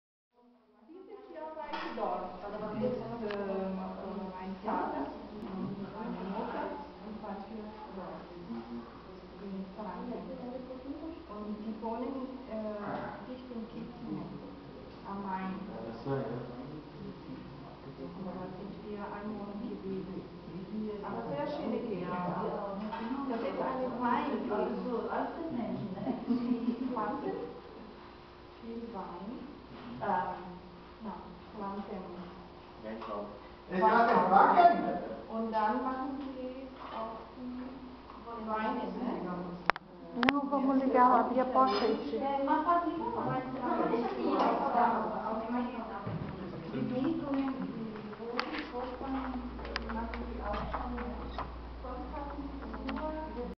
C.C. 25 de Julho de Blumenau - Original Altenberger Schuhplattler - Almoço
Almoço entre  integrantes do Original Altenber e integrantes do C.C. 25 de Julho,  Dia 14 de Outubro de 2011.